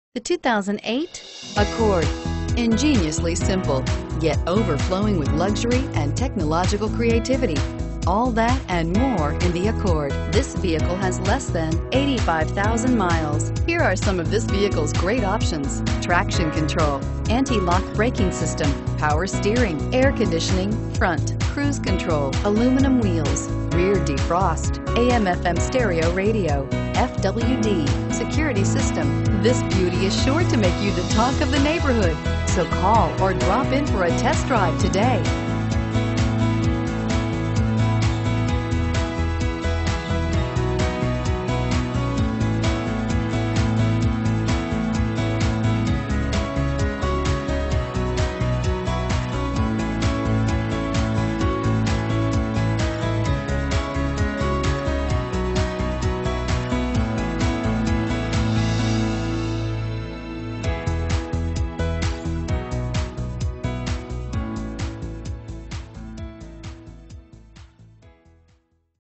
2008 Honda Accord Sdn Gainesville GA P9594
2008 Honda Accord Sdn LX-P

-NEW WIPER BLADES- -MP3 CD PLAYER, KEYLESS ENTRY, AND TIRE PRESSURE MONITORS- -GREAT GAS MILEAGE- This Accord Sdn looks great with a clean Black interior and Polished Metal Metallic exterior! Save money at the pump knowing this Accord Sdn gets 31.0 MPG! Please call to confirm that this Accord Sdn is still available! We are located at: 2420 Browns Bridge Rd, Gainesville, GA 30504.